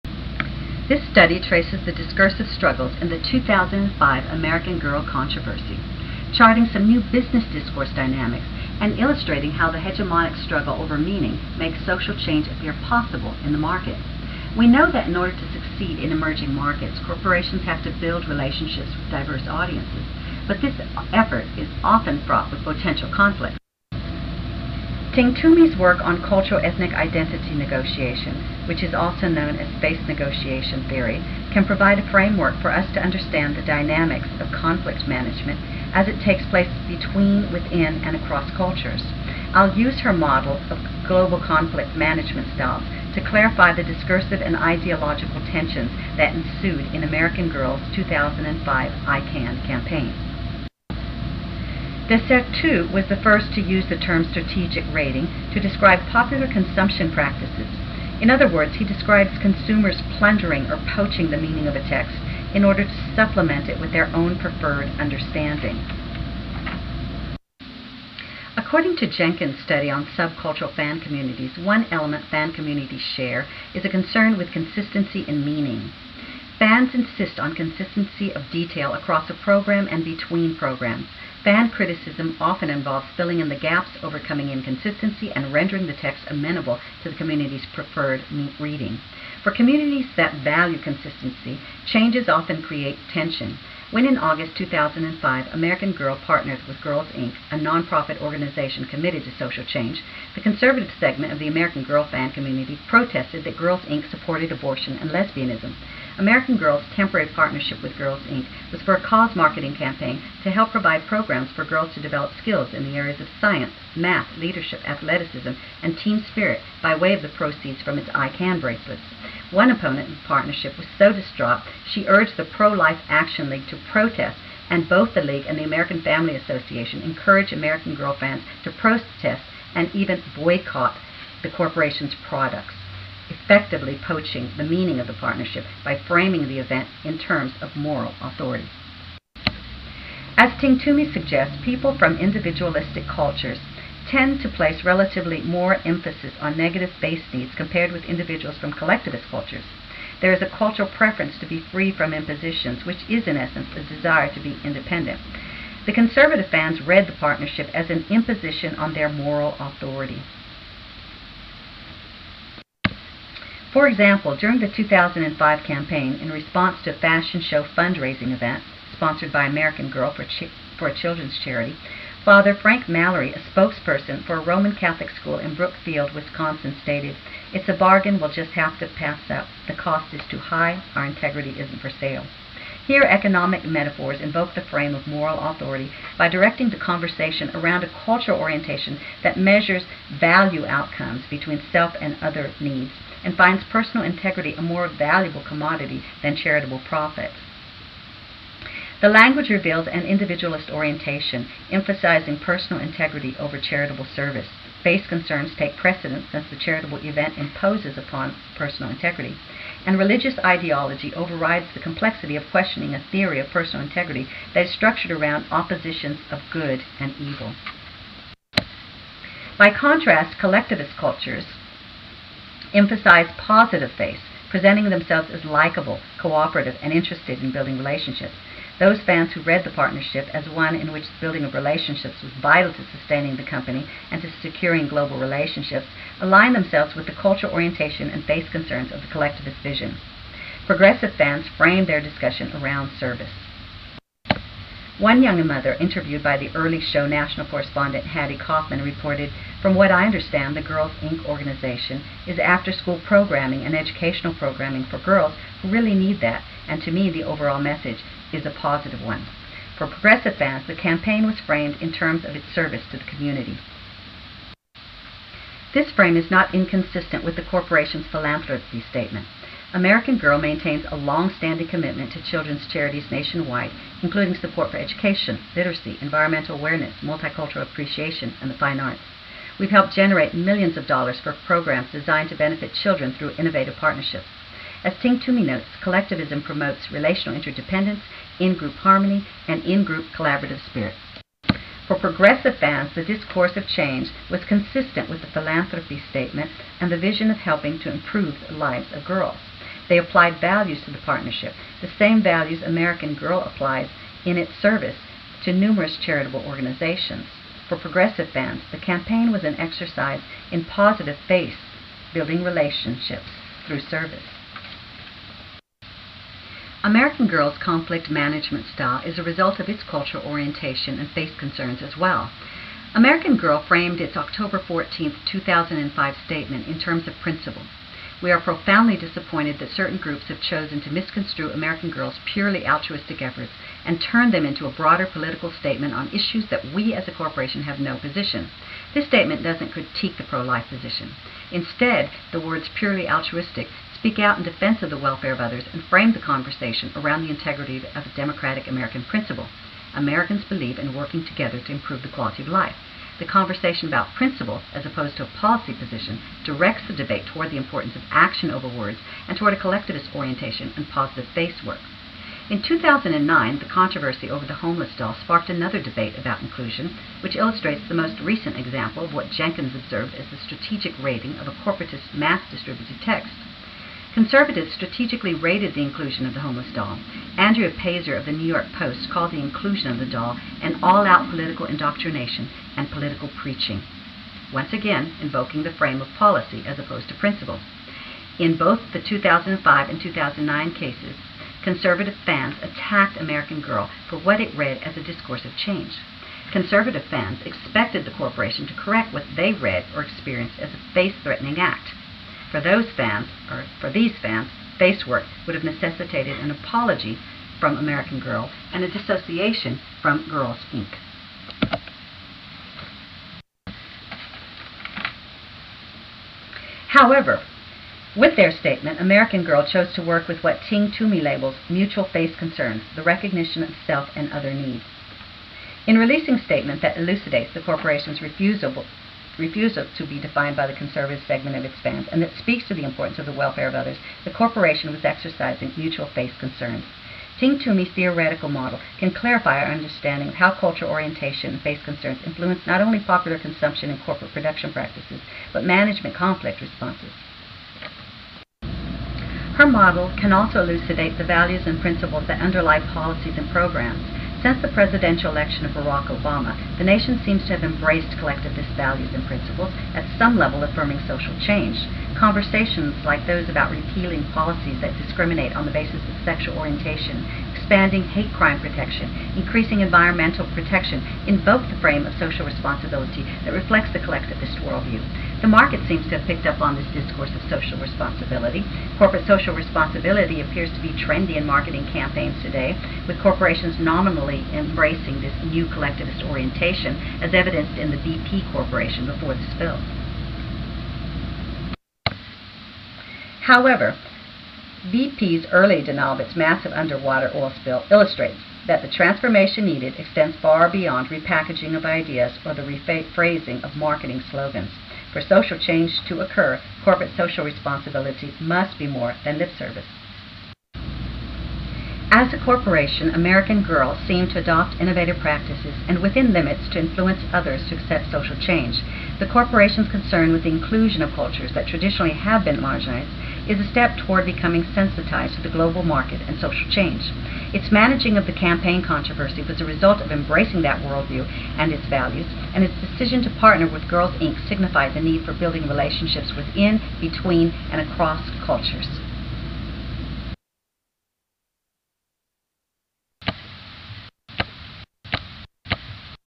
The Strategic Raiding of a Campaign Discousre Powerpoint.wmv
Yes, We Can" 2008 presidential campaign slogan of Barak Obama echoes an earlier promotion slogan by the American Girl campaign of 2005. The American Girl "I Can" campaign marked its beginnings with a discourse of change that necessarily brought challenges to its organization by interest groups who used the discourse of stability to offset any changes to the organization. This paper investigates the discourse of stability and change as it regards a corporation's relationship with its consumers. Using Michel de Certeau's (1974) concept of strategic raiding and Henry Jenkins' (1992) outline of the practices of fans as sub-cultural communities, this paper explores ways American Girl fans attempted to poach the meanings of the corporate campaign. A discourse of change often engenders challenges, but these challenges have more recently been trumped by a new developing culture of forward-looking innovators in industry. Using American Girl as one case study to trace the emerging discursive practices between a corporation and its consumers may help identify the ways hegemony as a continuous struggle over meaning plays a role in the marketplace that makes social change appear possible.

The business culture of today depends for its success on an integrated approach to business communication that embraces a global marketplace. Communicating with these emerging markets demands an interactivity that calls for inclusive strategies that reach out to diverse groups of cultures and subcultures. To illustrate how inclusive strategies stimulate new discursive and ideological tensions that can bring about social change, this case study draws on Stella Ting Toomey's cultural/ethnic identity negotiation theory. Like the "Yes, We Can" 2008 campaign of Barak Obama, the "I Can" 2005 campaign of American Girl resonated with this discourse of social change.